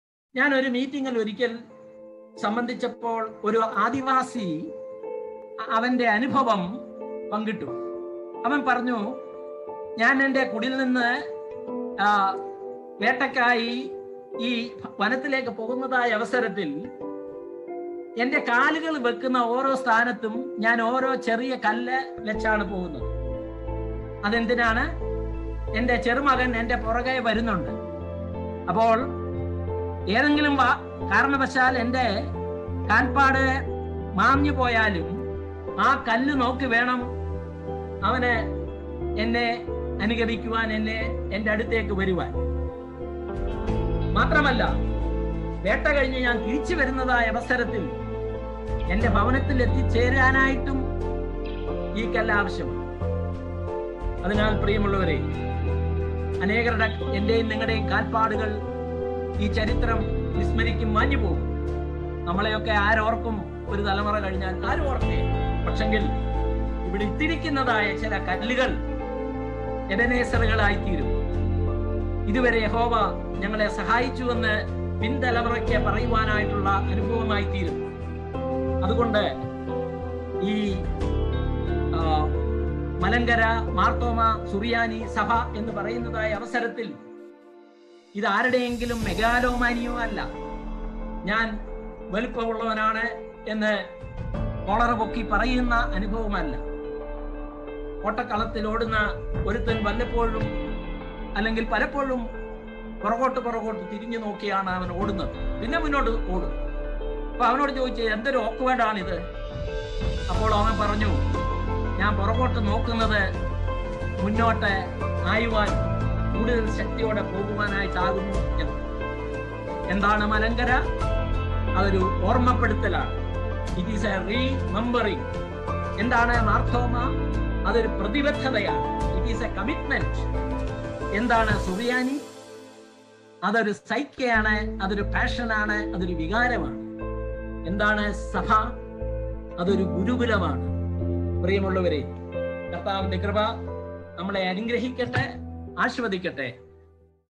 Church & Traditions I Rev.Dr R C Thomas I Mar Thoma Apologetics I Short Video 1
മലങ്കര മാർത്തോമാ സുറിയാനി സഭയിലെ പാരമ്പര്യ രീതികളുടെ പ്രാധാന്യത്തെപ്പറ്റി Rev.Dr R C Thomas
അച്ചൻ വിവരിക്കുന്നു.

Taken from the Webniar Session -1 (Know our Church )